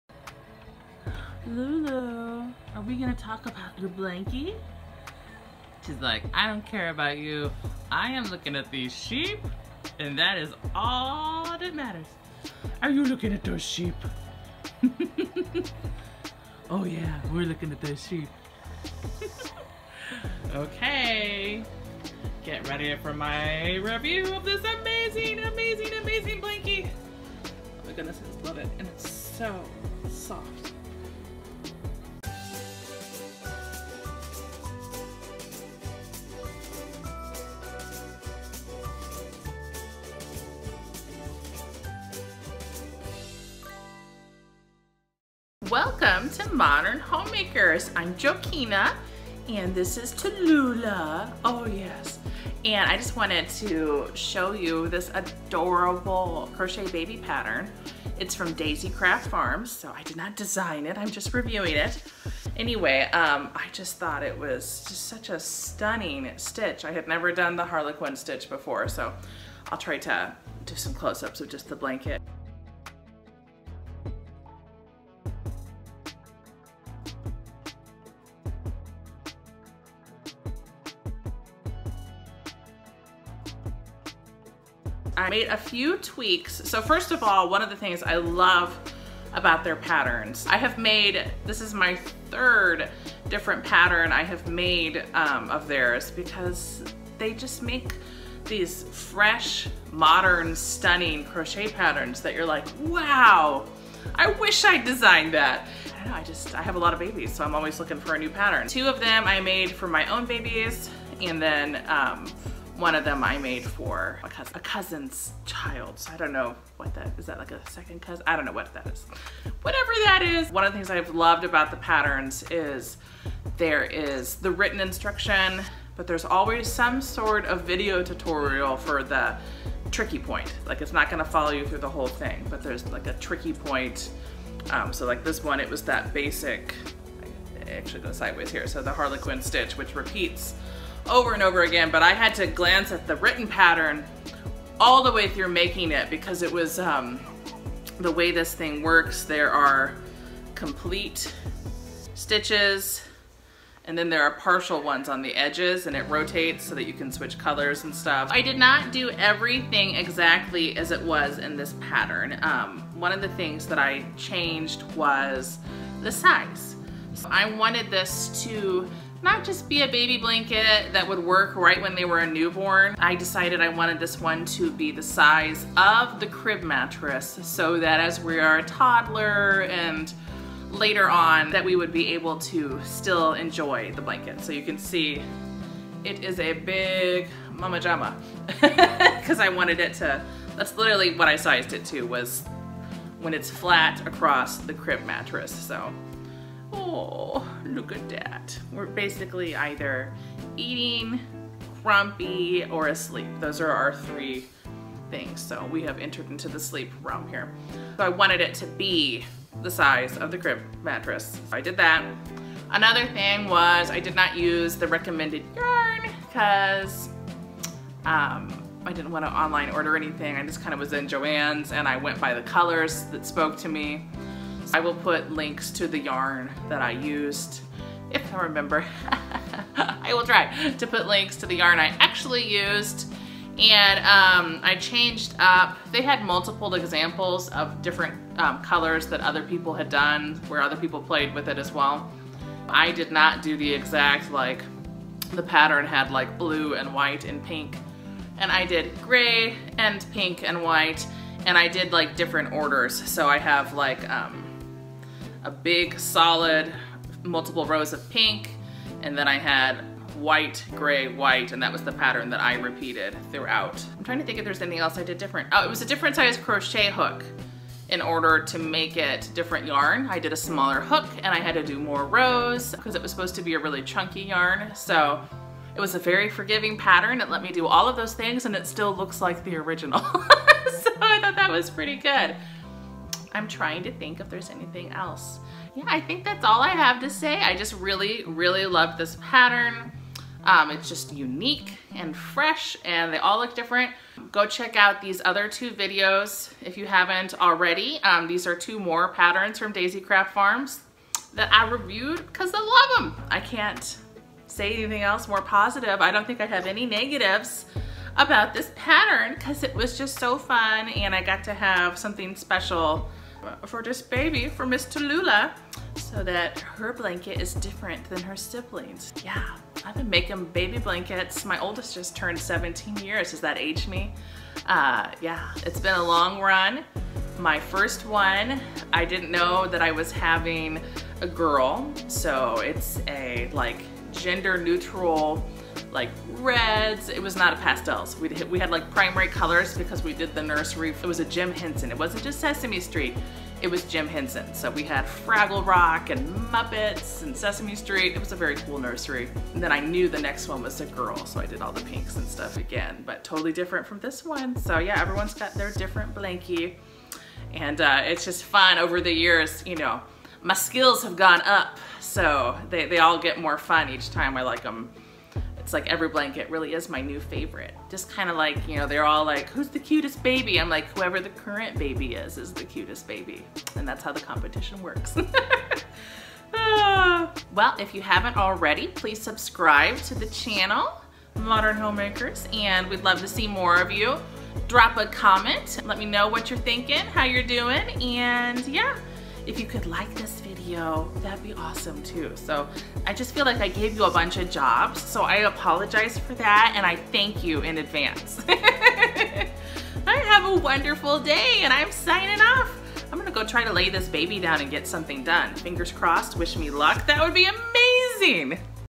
crochet baby pattern review daisy craft farms harlequin blanket
This was such a special project for me, making the crochet blanket for my fifth baby :-) I highly recommend this pattern. It's free, which is my favorite price and you can find it here:

It was one of the cheapest baby blankets I've ever made, so I highly recommend trying out this yarn instead of a more expensive baby yarn. However, a natural cotton or bamboo would be even better if that's in your budget :-)